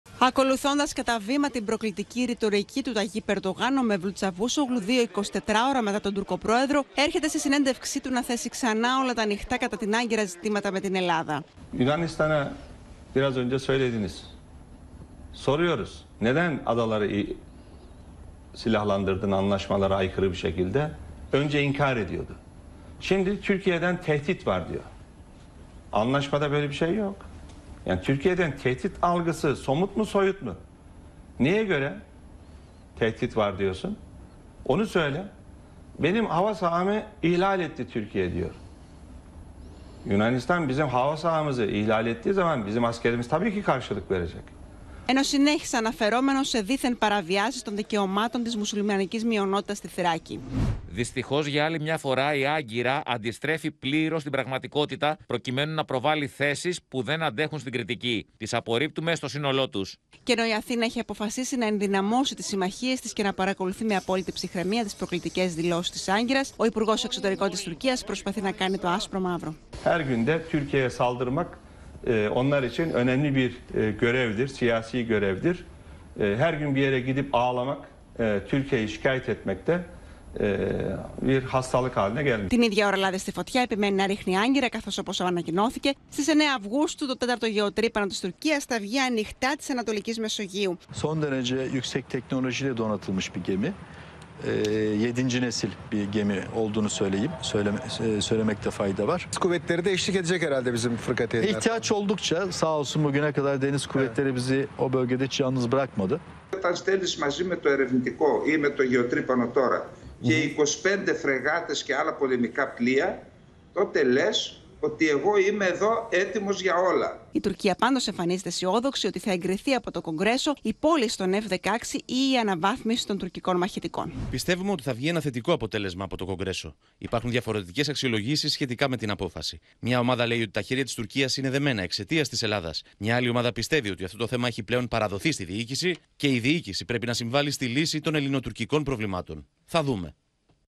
Κρεσέντο προκλητικότητας Τσαβούσογλου κατά της Ελλάδας | Κεντρικό Δελτίο Ειδήσεων | OPEN TV
Γίνετε μέλος στο κανάλι του OPEN για να αποκτήσετε πρόσβαση σε προνόμια:
Νέο κρεσέντο προκλητικότητας από την Τουρκία. Αυτή τη φορά ο τούρκος ΥΠΕΞ Μεβλούτ Τσαβούσογλου επιτίθεται στην Ελλάδα, την οποία κατηγορεί ότι δήθεν παραβιάζει τα δικαιώματα της μουσουλμανικής μειονότητας στη Θράκη. Την ίδια ώρα η Αθήνα παρακολουθεί κάθε κίνηση της Άγκυρας σε Αιγαίο και Μεσόγειο, καθώς η Τουρκία ανακοίνωσε πως στις 9 Αυγούστου αναμένεται να βγει το 4ο γεωτρύπανο στην ανατολική Μεσόγειο.